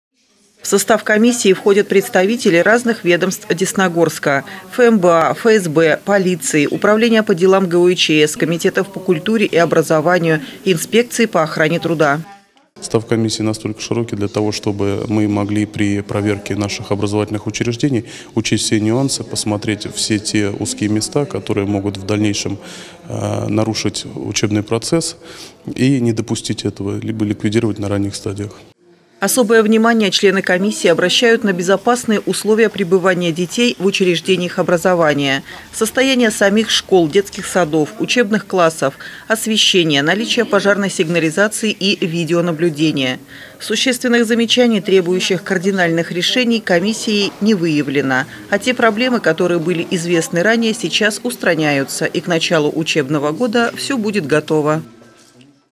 Десна-ТВ: Комиссия проверяет готовность образовательных учреждений к началу учебного года
На этой неделе  в городе работает  межведомственная комиссия по приёмке учреждений образования к началу учебного года.